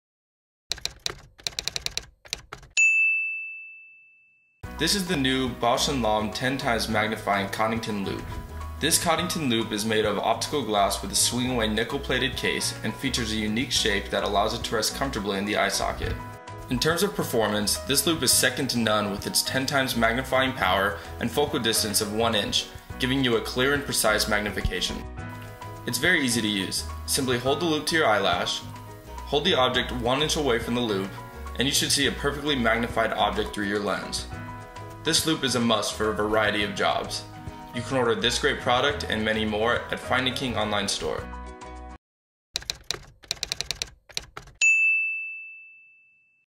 10x Bausch & Lomb Coddington Loupe
This is a new Bausch & Lomb 10X watchmaker's loupe

This is a single lens loupe made of optical glass

It has a swing-away, nickel plated case

It's shape allows it to rest comfortably in the eye socket

Provides sharp, crisp images

The lens measures approximately 20 mm (13/16") in diameter with a

1" (25 mm) focal distance